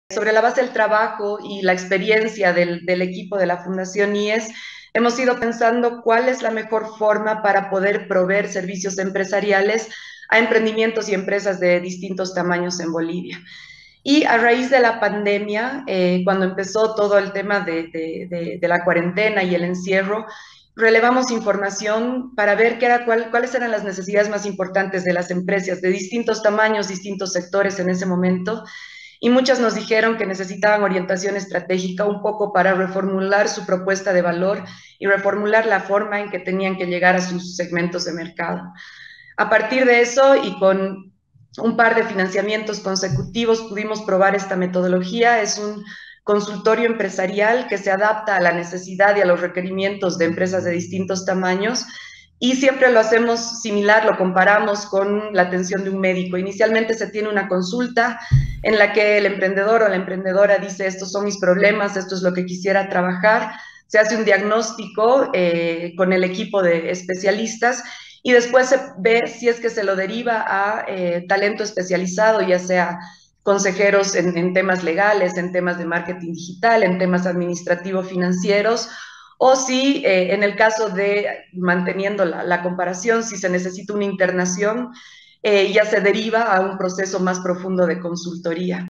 Inversiones de impacto en Bolivia - Lucía Casanovas
La experta dijo que el IES viene brindando sus consultorios empresariales desde hace 2 años: “es la mejor forma para proveer servicios a empresas de distintos tamaños en Bolivia. A raíz de la pandemia relevamos información y los emprendedores nos dijeron que necesitaban orientación estratégica para reformular su propuesta de valor y la forma en que tenían que llegar a sus segmentos de mercado. Inicialmente, se tiene una consulta donde el emprendedor dice cuáles son sus problemas y lo que quisiera trabajar. Se hace el diagnóstico con un equipo de especialistas y después se ve si se lo deriva a un talento especializado: consejeros en temas legales, marketing digital y temas administrativo-financieros, o si se necesita un proceso más profundo de consultoría”.